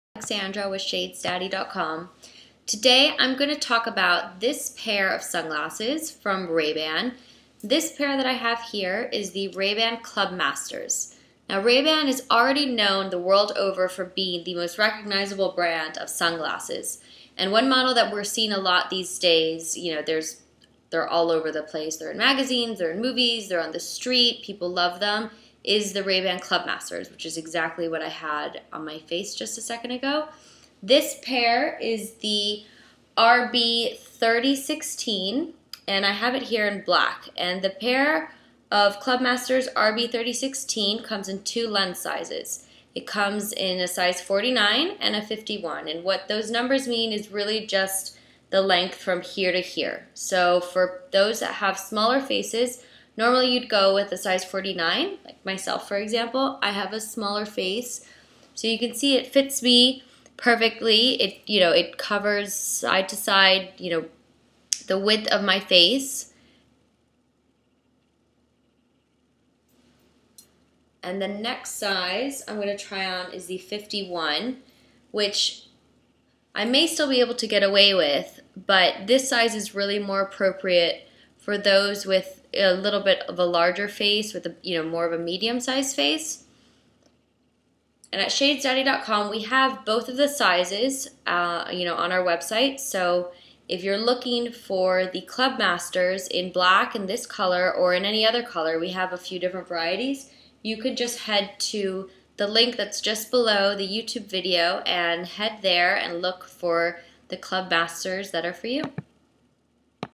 Ray-Ban Clubmaster Sunglasses RB3016 Size Comparison: 49mm and 51mm
Review of the Ray-Ban Clubmaster RB3016 sizes.

Size Review:

49mm: Small to Medium Fit
51mm: Medium to Large Fit

The Ray-Ban Clubmaster RB3016 is currently being made in China. To secure and guarantee authenticity of these sunglasses, shop at ShadesDaddy.com